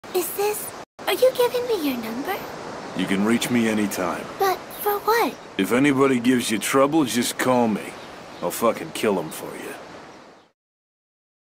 [OMORI AU] sunny being overprotective for no apparent reason // shitpost + desc
i was gonna make this video take place inside the orphanage that omori was in but i didn't have enough slots to make background characters and i feel like it would be weird for it to take place in an empty orphanage, so you're getting an empty brick wall 💀

sunny is giving omori one of mari's old phones btw

i still don't have a name for this au 💀💀

i don't have this au made in gl2 yet so i had to use gacha club

basic run-down of this au that i'll (probably) be putting in the desc of every video i make with it:

sunny was always a bit of a recluse
mari never ordered pizza
omori is a real person, specifically the adopted sibling of sunny and mari
basil and aubrey switch personalities, but basil is still the gardener of the group
kel is still extremely energetic, but he's slightly more open about his feelings